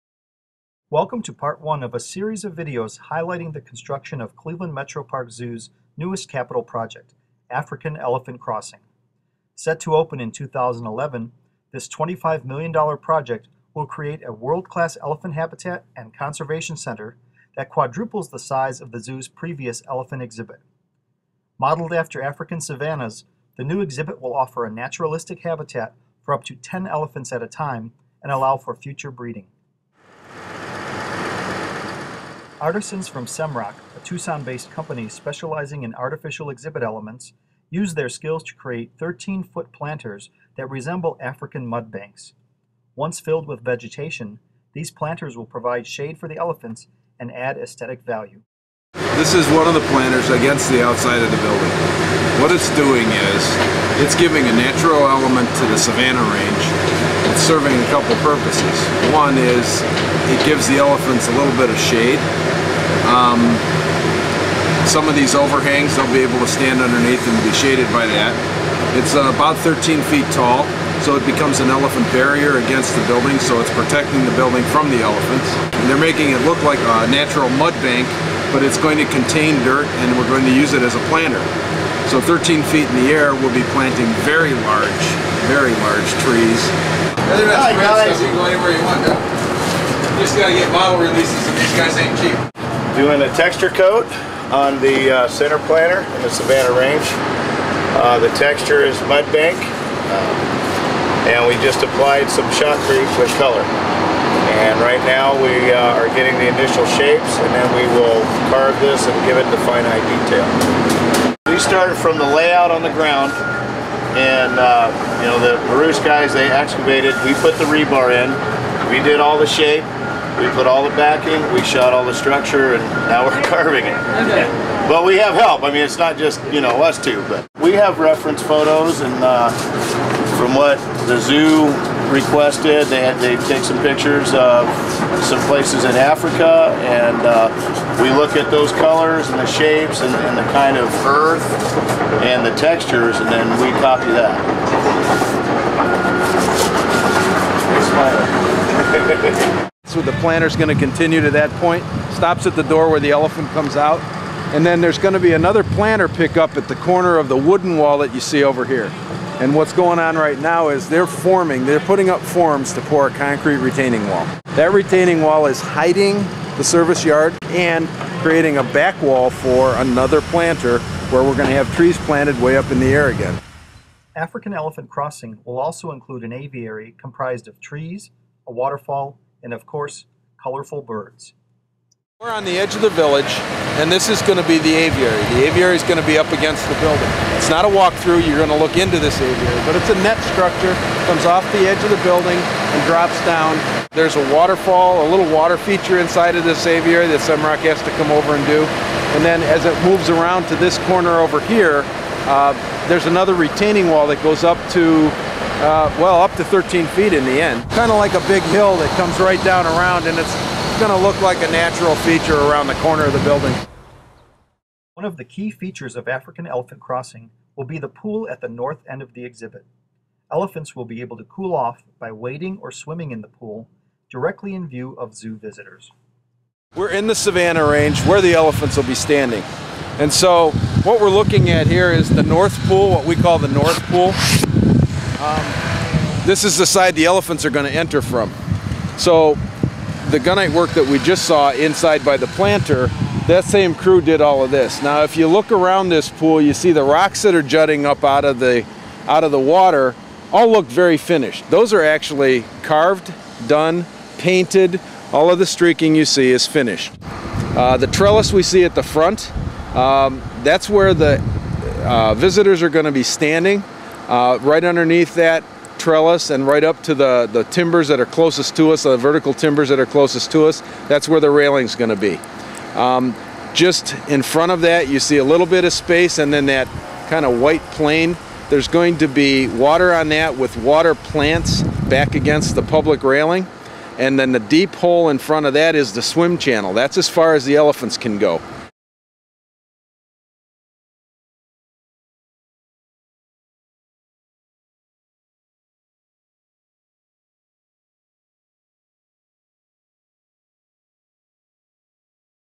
African Elephant Crossing Construction Update Part 1
As Cleveland Metroaparks Zoo takes you behind the scenes to meet some of the people responsible for African Elephant Crossing and give you a glimpse of what it takes to build a world-class exhibit. In this construction update, you'll learn about some of the external features in the exhibit's "Savanna Range" -- huge mud-bank planters, an outdoor aviary and the north elephant pool.